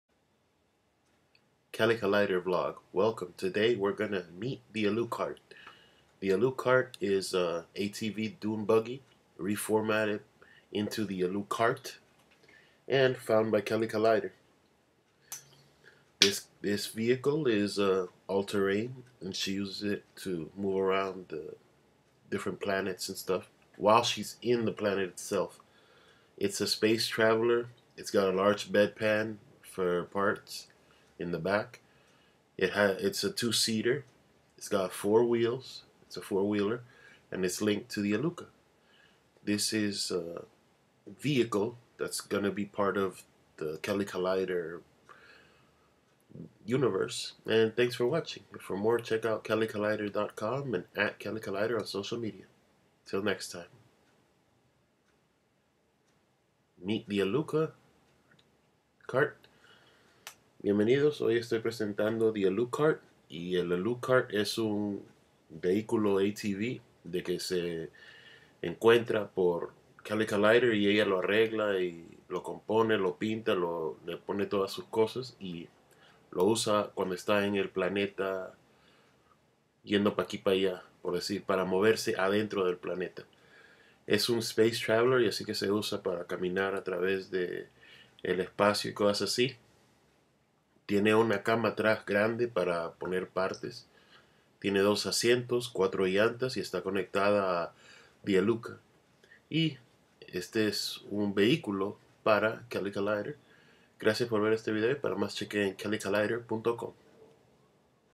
Kelly Colider vlog 18 Meet The Alukart
Kelly Colider vlog 18 Introducing The Alukart dune buggy vehicle.
Kelly Colider vlog 18 presentando The Alukart vehiculo.